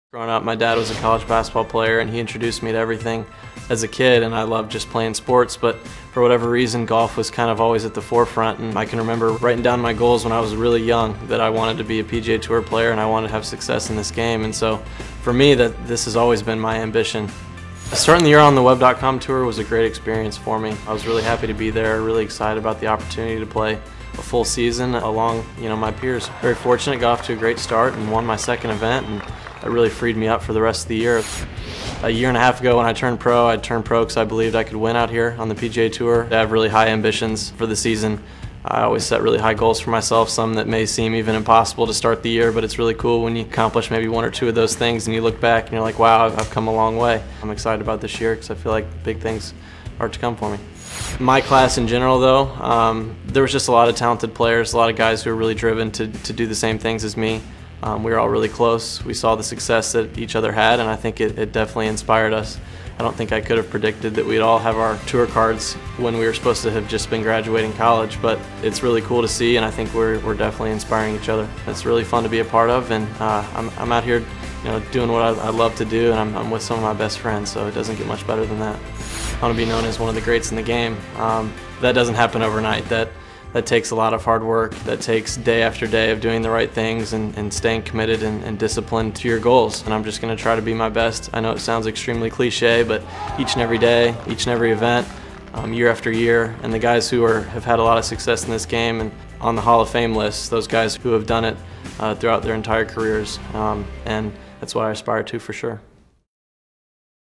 Getting to know Patrick Rodgers - PGA TOUR Rookie
After playing 17 PGA TOUR events in 2015, Patrick Rodgers reflects on his Web.com experience and what his expectations are going in to his first full-time season on TOUR.